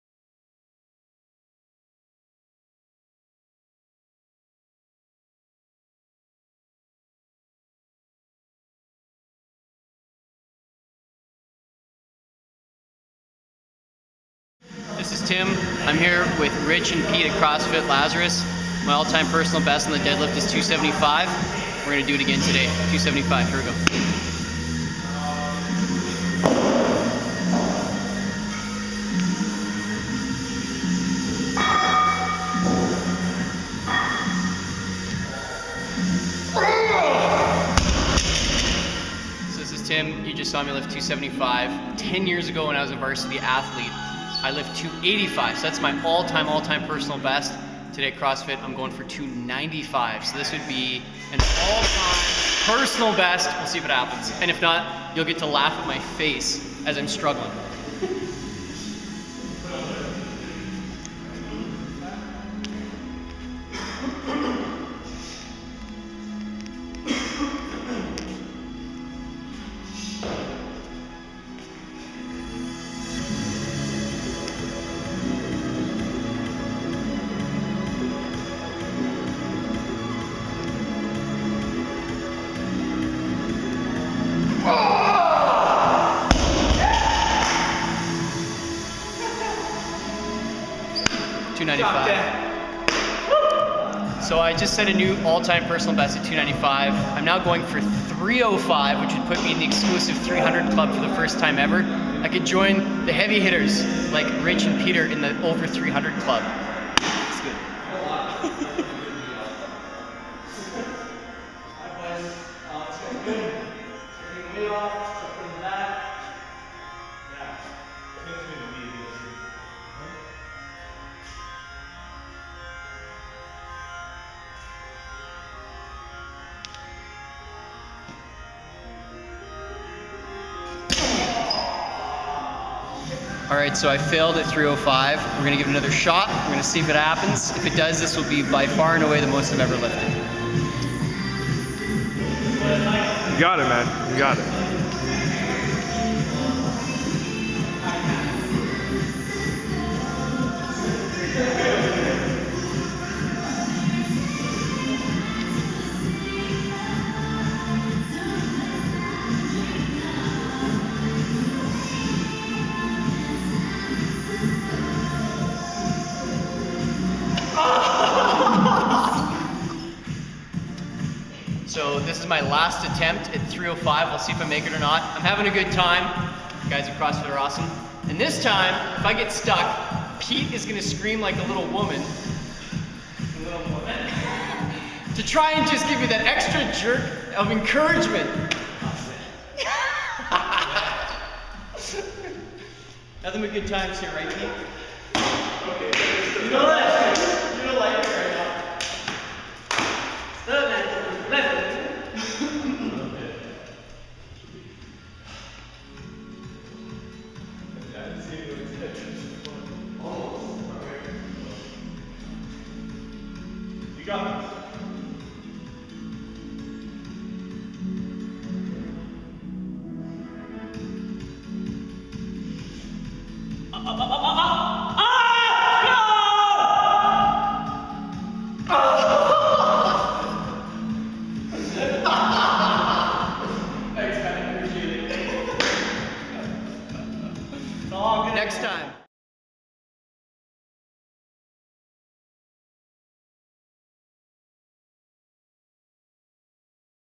Tim Francis deadlifting
Tim Francis a athlete at CrossFit Lazarus working to beat his previous deadlift record set during the May deadlifting competition at the gym.

CrossFit Lazarus is located in Edmonton, ALberta, Canada on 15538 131 ave.